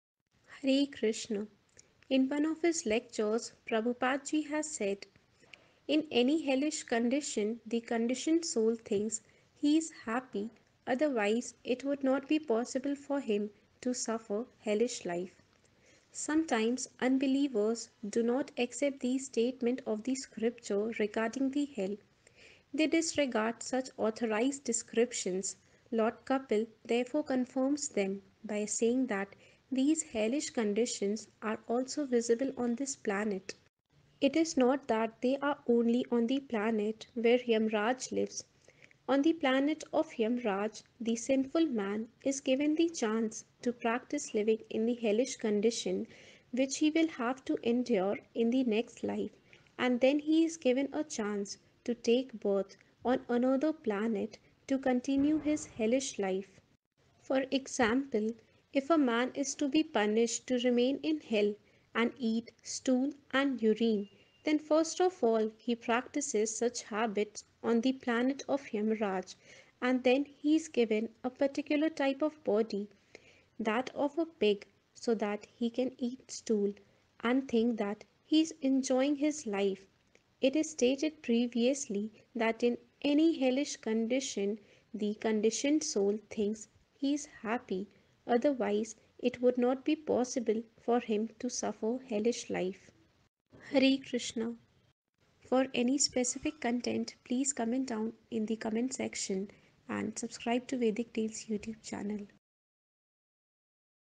Why Few People SUFFER HELLISH LIFE on Earth,
Prabhupada Speaks on, IN ANY HELLISH CONDITION THE CONDITIONED SOUL THINKS HE IS HAPPY; OTHERWISE IT WOULD NOT BE POSSIBLE FOR HIM TO SUFFER HELLISH LIFE.

Sometimes unbelievers do not accept these statements of scripture regarding hell. They disregard such authorized descriptions. Lord Kapila therefore confirms them by saying that these hellish conditions are also visible on this planet. It is not that they are only on the planet where Yamarāja lives. On the planet of Yamarāja, the sinful man is given the chance to practice living in the hellish conditions which he will have to endure in the next life, and then he is given a chance to take birth on another planet to continue his hellish life. For example, if a man is to be punished to remain in hell and eat stool and urine, then first of all he practices such habits on the planet of Yamarāja, and then he is given a particular type of body, that of a hog, so that he can eat stool and think that he is enjoying life. It is stated previously that in any hellish condition, the conditioned soul thinks he is happy. Otherwise, it would not be possible for him to suffer hellish life.